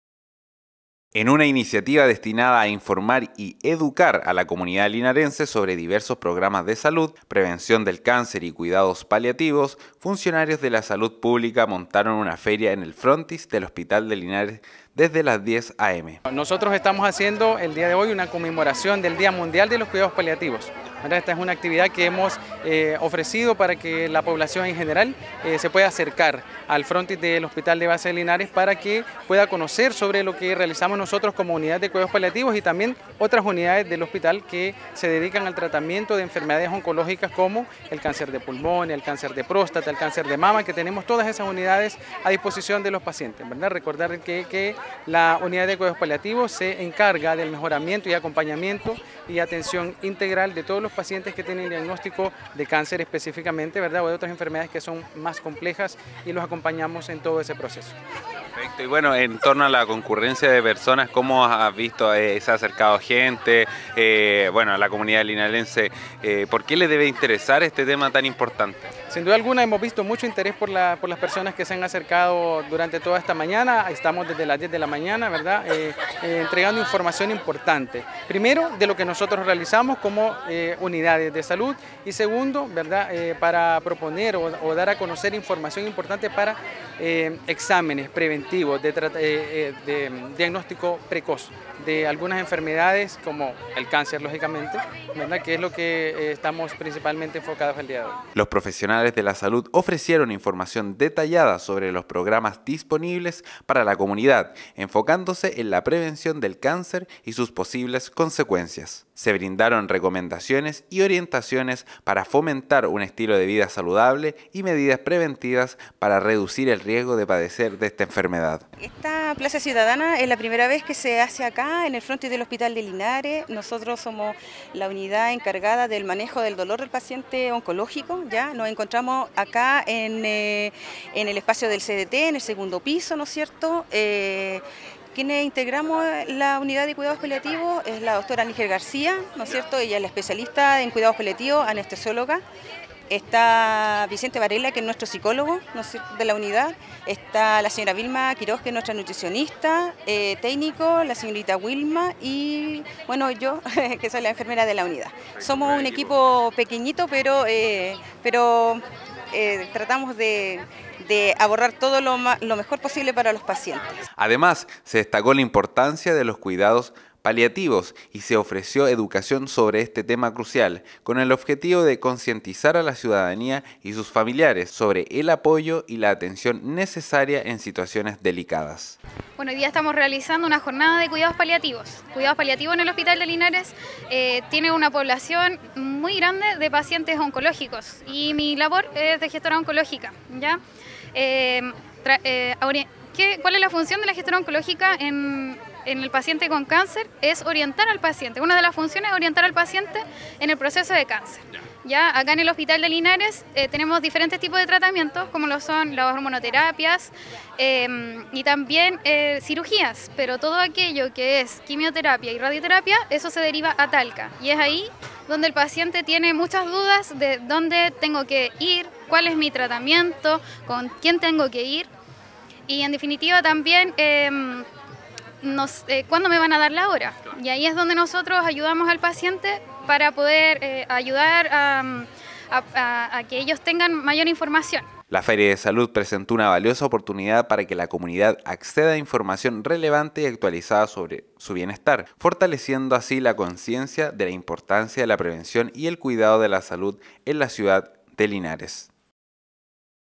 Feria Salud Hospital Linares | Viernes 13.10.23
En una iniciativa destinada a informar y educar a la comunidad linarense sobre diversos programas de salud, prevención del cáncer y cuidados paliativos, funcionarios de la salud pública montaron una feria en el frontis del Hospital de Linares desde las 10:00 AM.